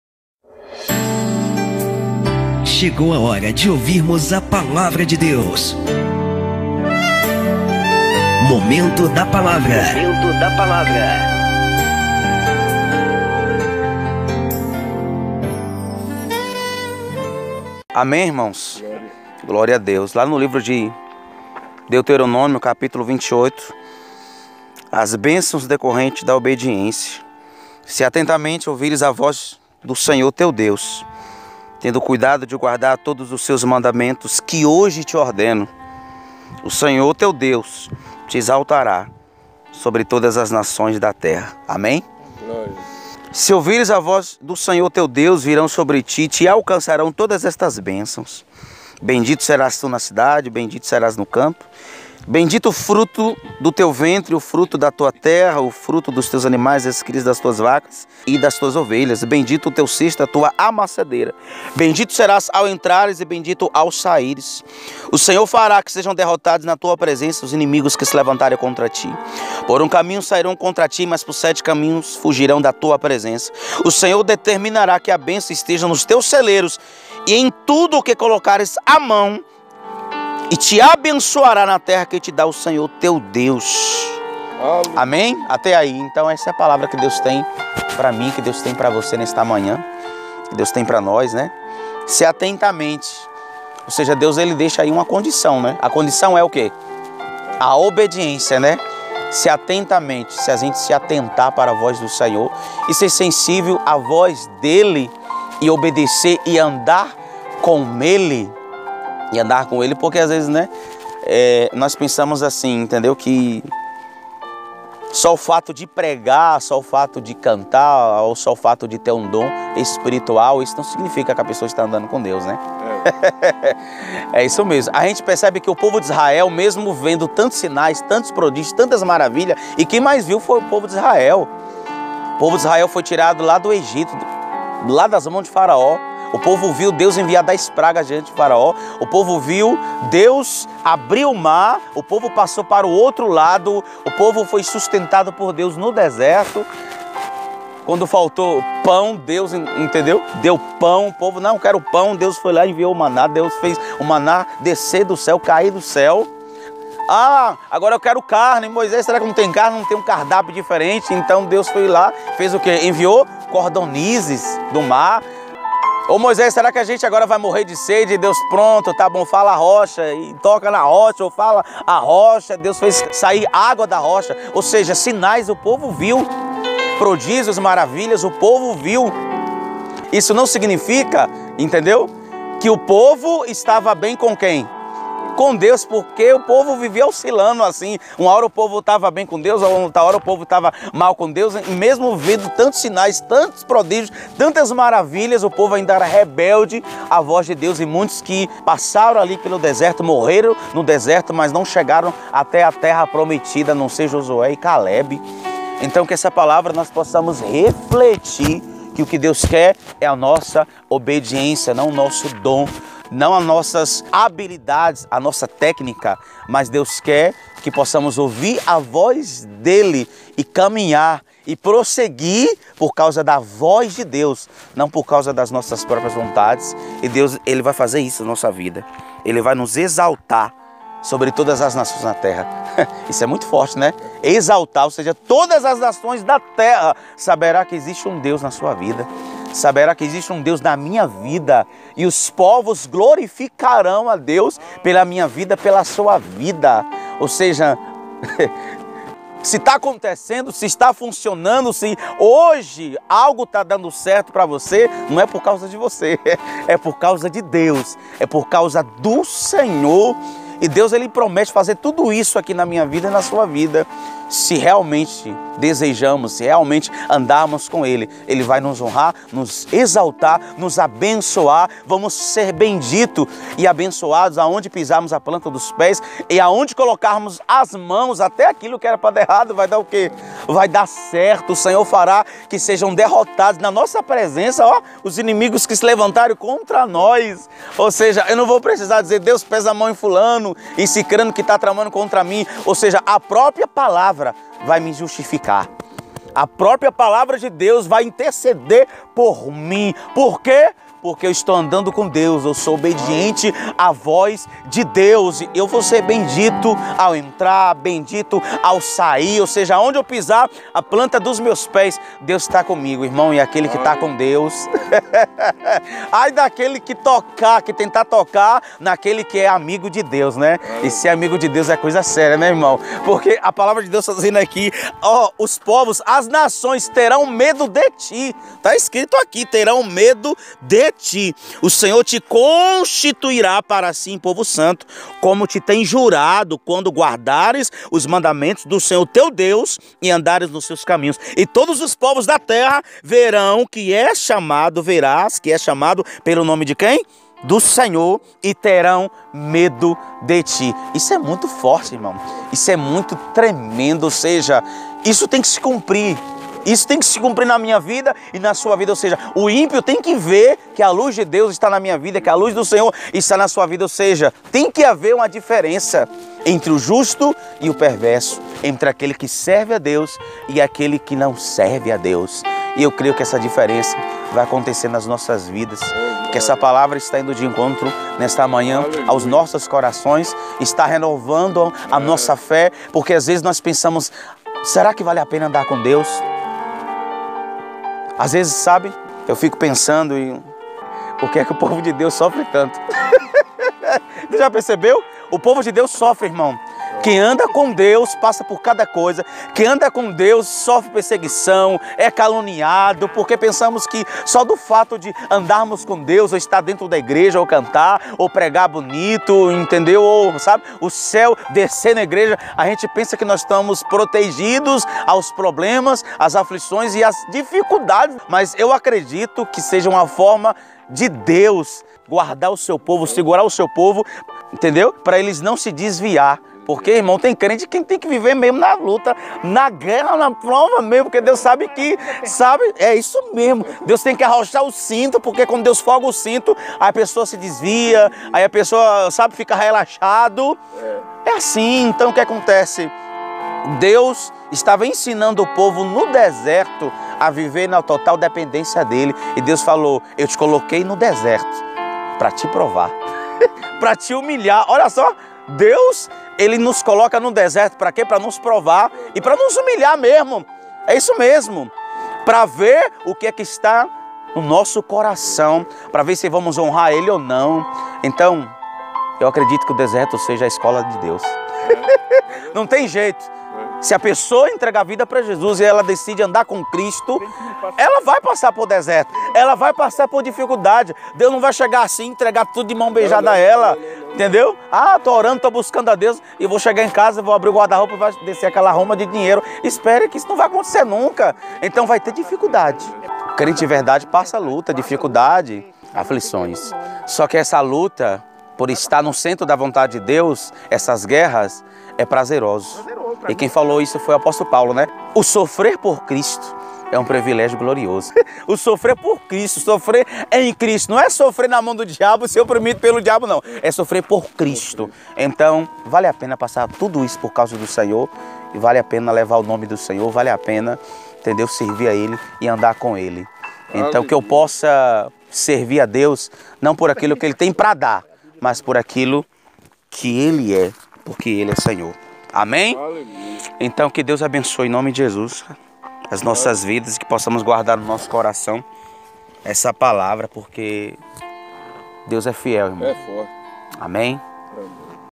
BÊNÇÃO OU MALDIÇÃO? (DEUT.28) MENSAGEM COM O PR.SIDNEY PEQUI
DO LEVITA PR.SIDNEY PEQUI👇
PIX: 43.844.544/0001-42